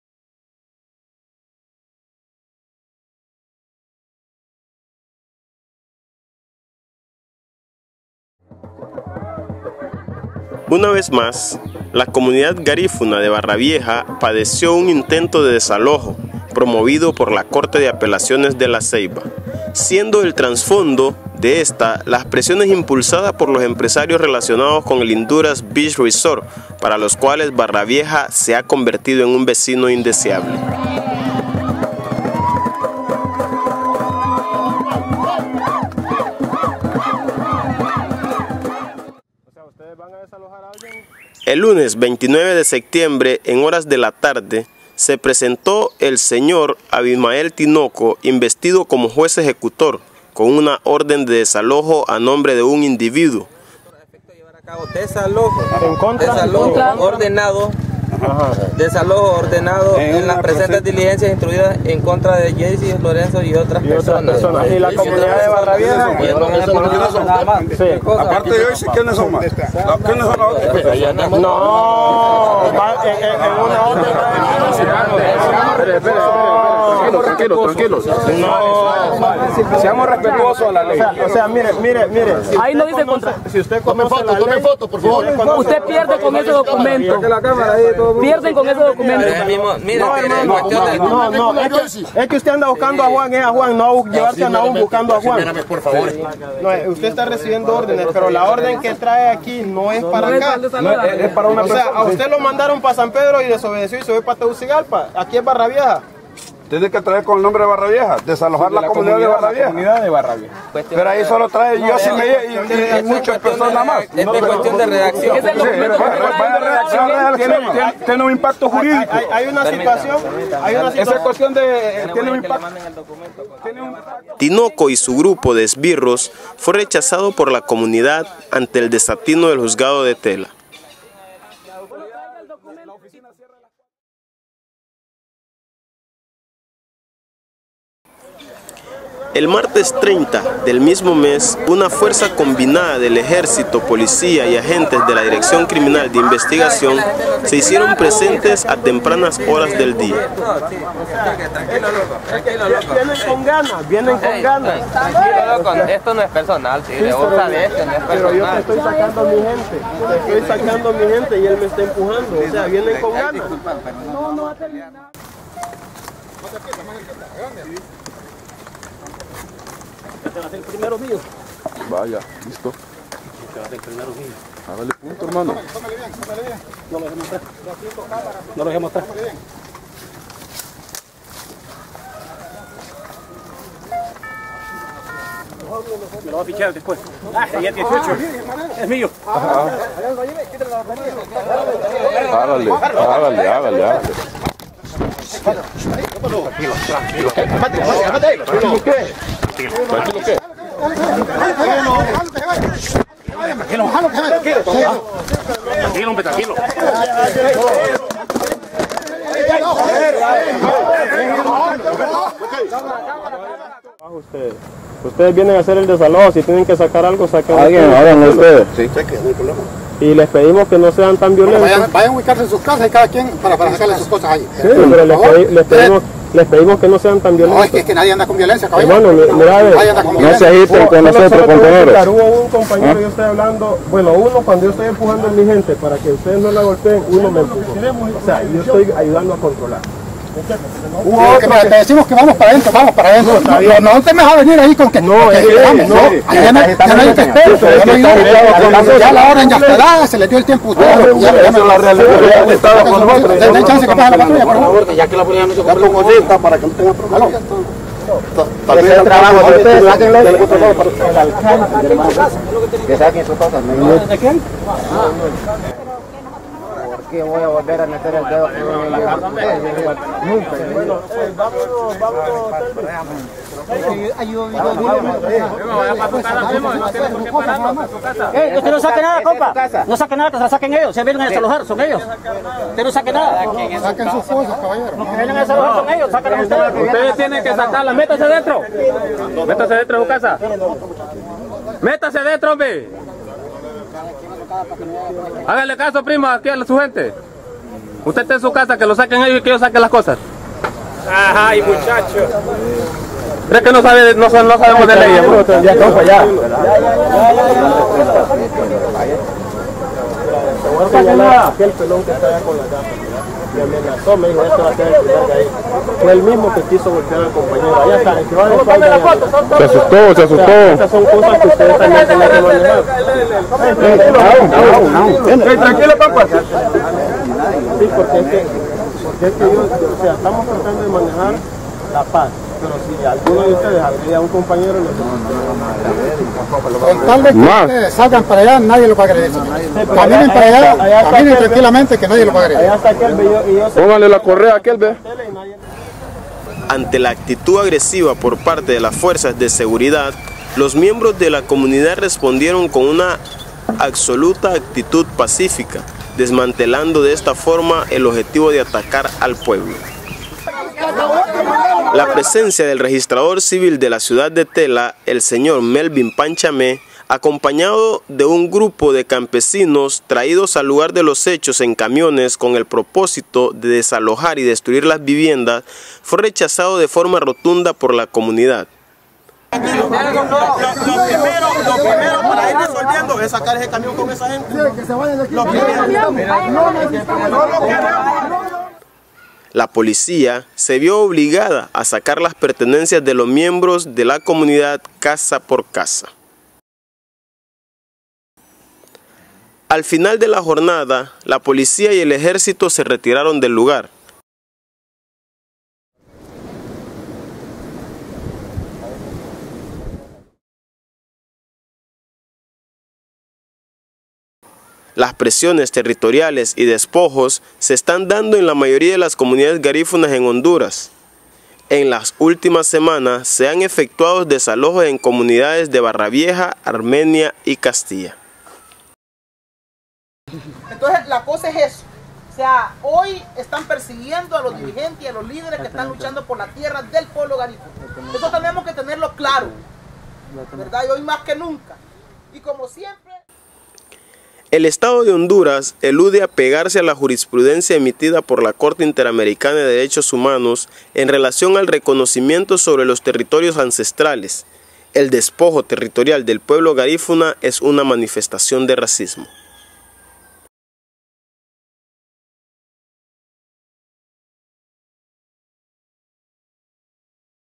EI Indura Resort y los desalojos en la comunidad Garífuna de Barra Vieja
Desalojo de la comunidad de Barra Vieja, el Indura Resort y la estrategia de expulsión de los Garífunas de Honduras.

El pasado 29 y 30 de septiembre, una fuerza combinada de policía y ejercito efectuaron un desalojo de la comunidad garífuna de Barra Vieja, Tela, procediendo a destruir las humildes casas de habitación y dispersar a sus moradores.

Barra Vieja está ubicado contiguo al proyecto turístico Indura Resort, conocido previamente como Laguna de Micos & Beach Resort, perteneciente a la elite de poder de Honduras, implicados directamente en el golpe de estado del año 2009. El proyecto fue inaugurado en noviembre del año pasado, al mismo tiempo que se efectuó una reunión del Consejo Empresarial de América Latina (CEAL). leer mas